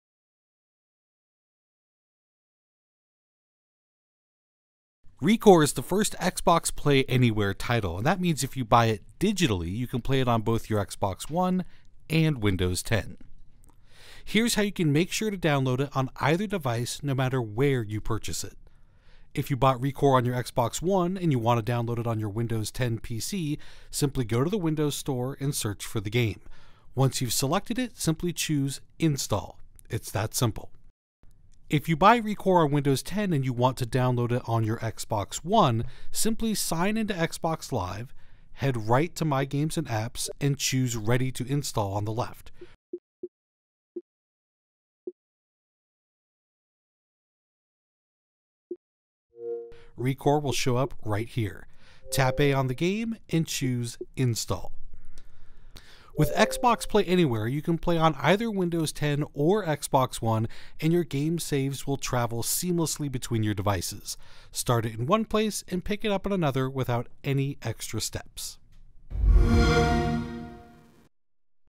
How to Download Your Xbox Play Anywhere Games
This quick tutorial shows you how to download Xbox Play Anywhere games, no matter what device you purchased them on.